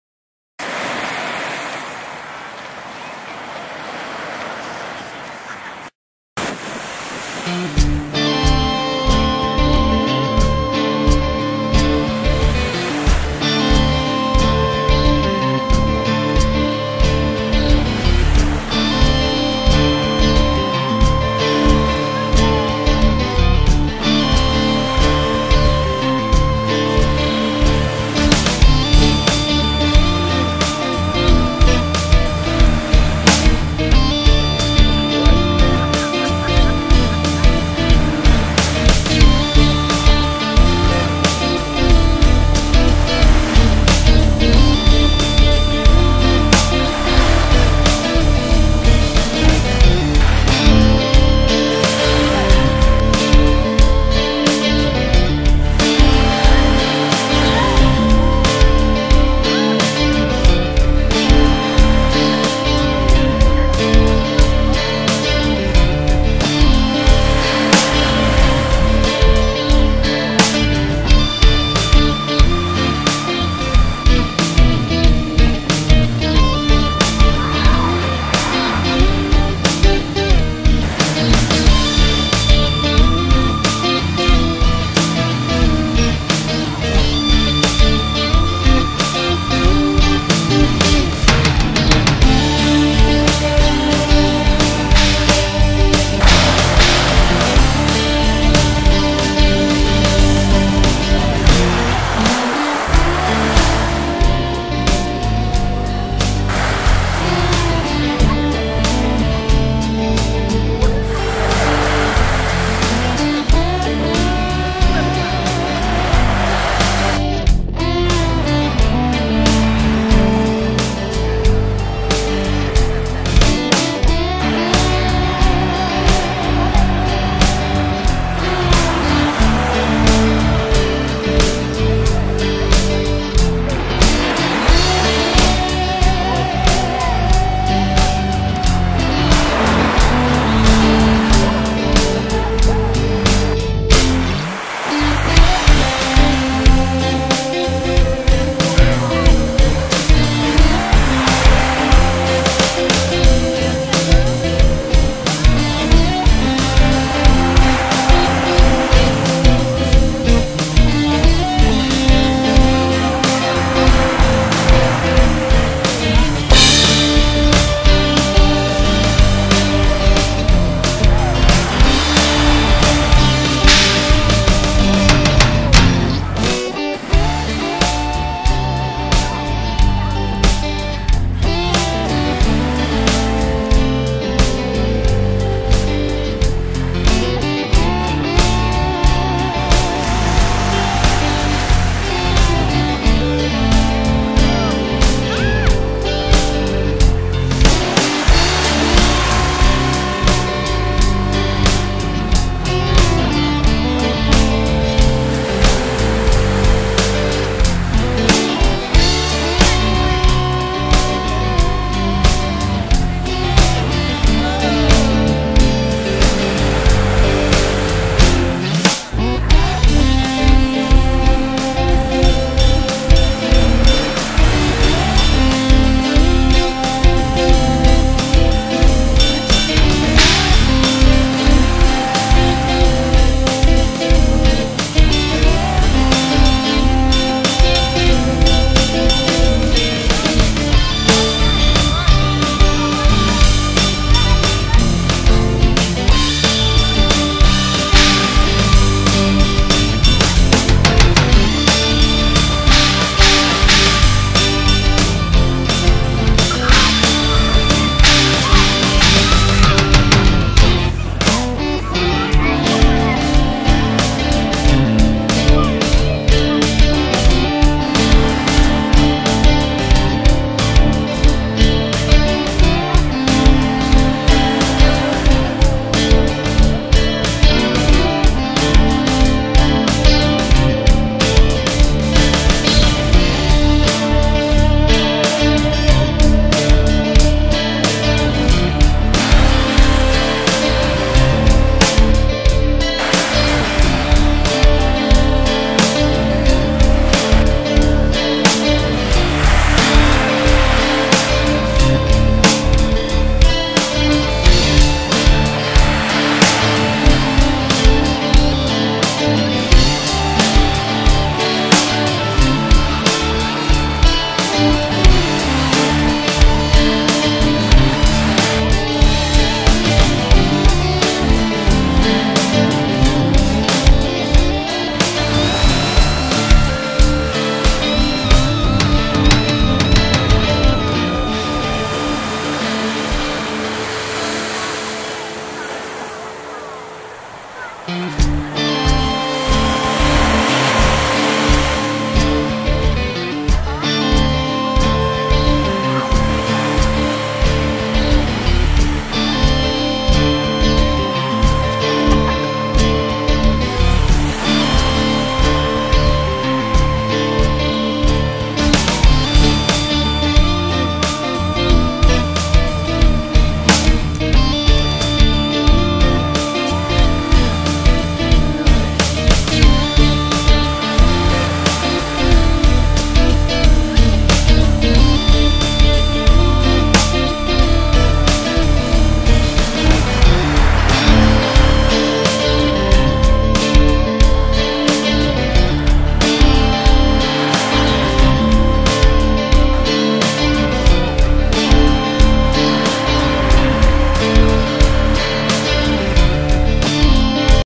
Sunset at Sunset
July 7th, 2013 ...North Shore Oahu.. with my honey !